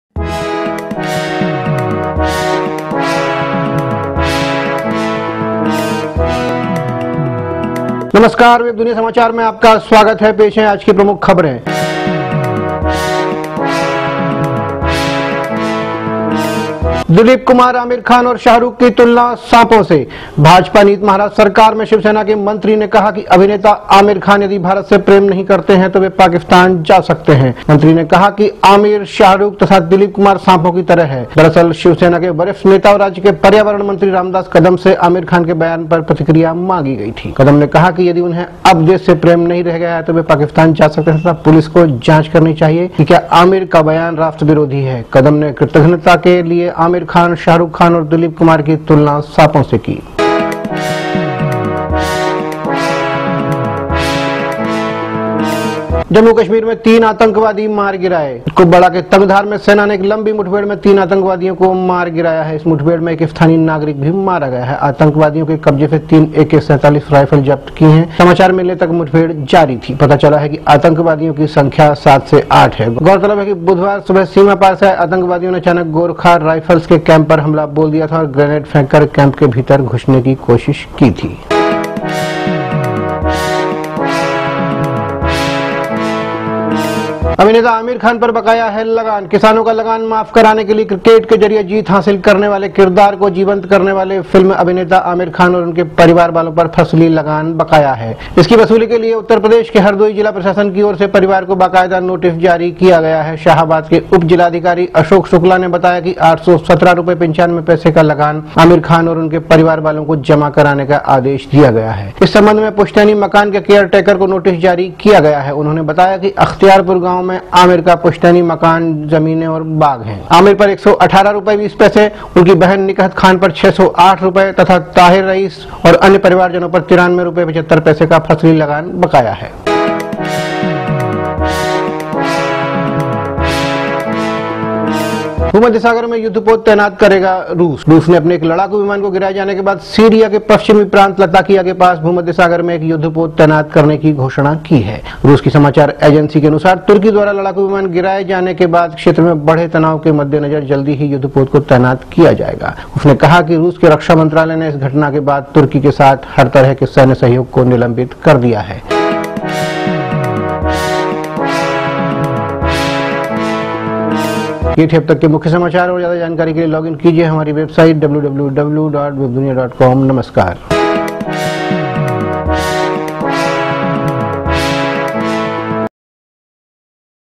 आमिर खान पर बकाया है लगान : Aamir Khan, family Owe Rs.817.95 as Land Rent
किसानों का लगान माफ कराने के लिए क्रिकेट के जरिए जीत हासिल करने वाले किरदार को जीवंत करने वाले फिल्म अभिनेता आमिर खान और उनके परिवार वालों पर फसली लगान बकाया है। इसकी वसूली के लिए उत्तर प्रदेश के हरदोई जिला प्रशासन की ओर से परिवार को बाकायदा नोटिस जारी किया गया। शाहाबाद के उपजिलाधिकारी अशोक शुक्ला ने बताया कि 817 रुपए 95 पैसे का लगान आमिर खान और उनके परिवार वालों को जमा कराने का आदेश दिया गया है।